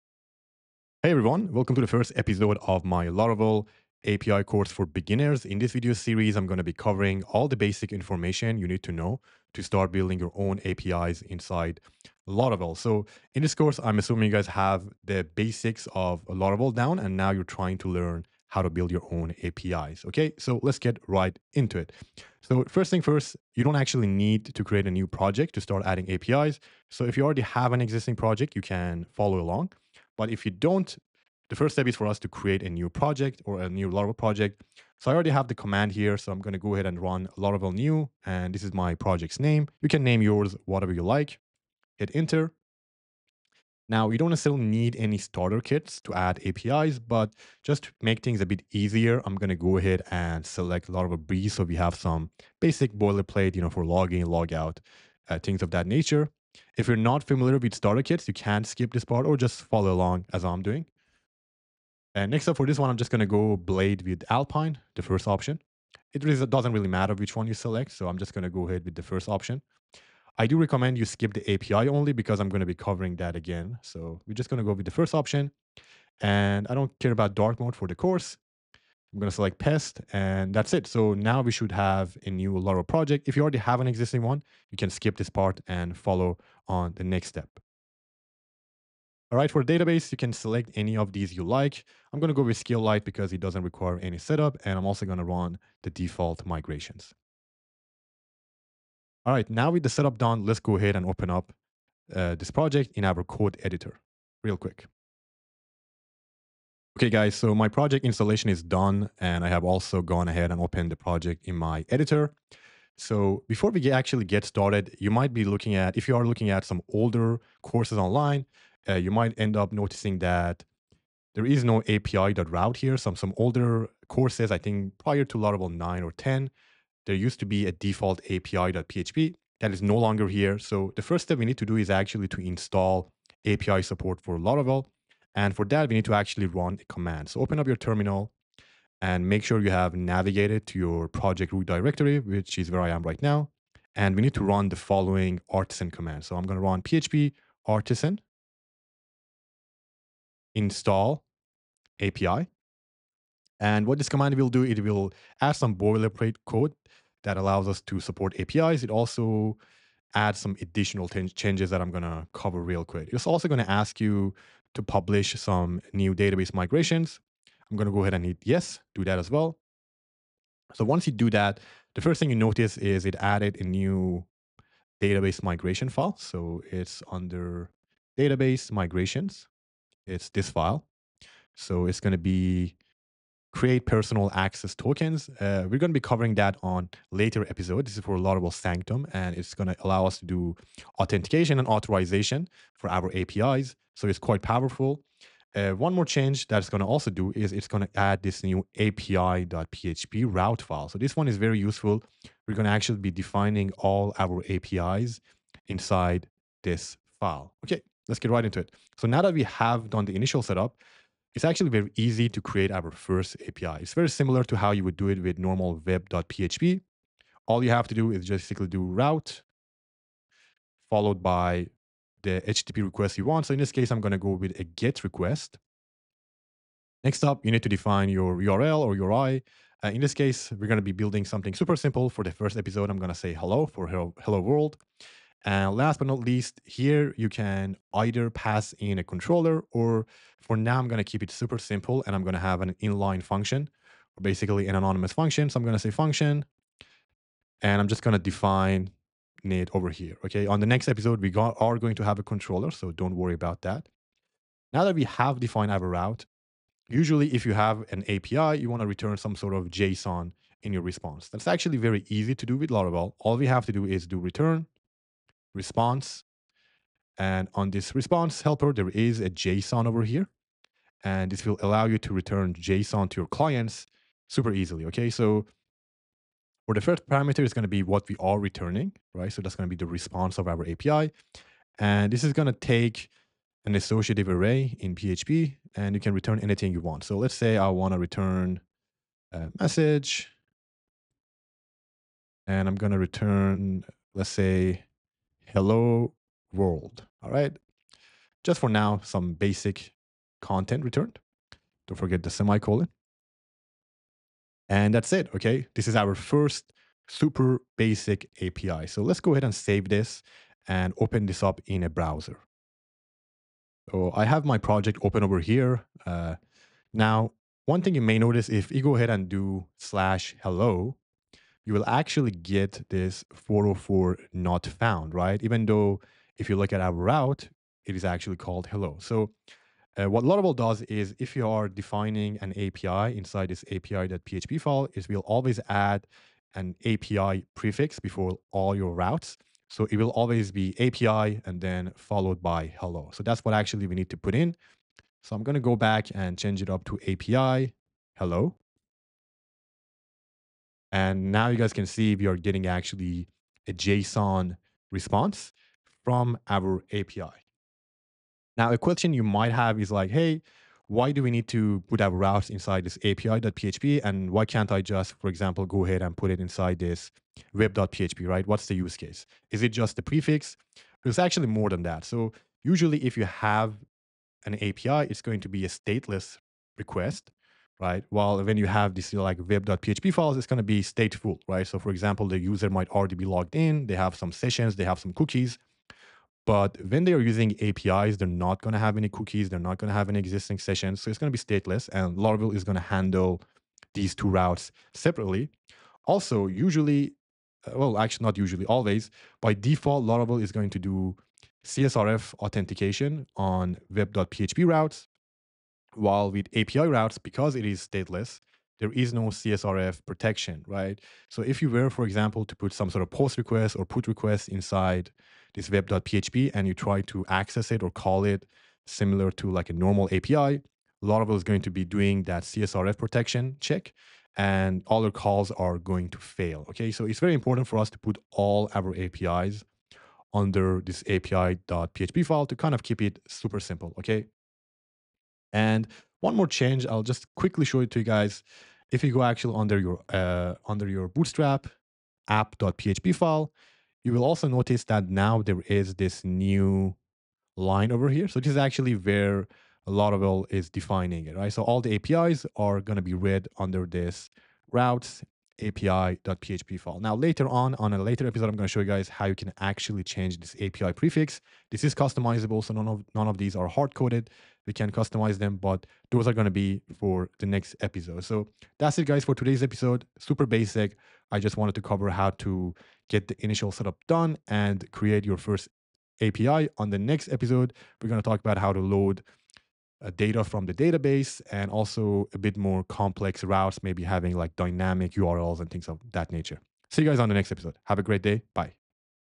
Your first API | Laravel API for beginners #1
If you’re new to APIs or feel confused about how backend and frontend apps talk to each other, this video is for you. We’ll start with how Laravel handles API routes, and why APIs live in api.php instead of web.php.

By the end of this course, you’ll have a real, working API endpoint that returns JSON — and you’ll understand exactly what’s happening, without any unnecessary theory.

What you’ll learn in this episode:

- What an API is (in simple terms)

- The difference between web.php and api.php

- How Laravel API routing works

- How to create your first API endpoint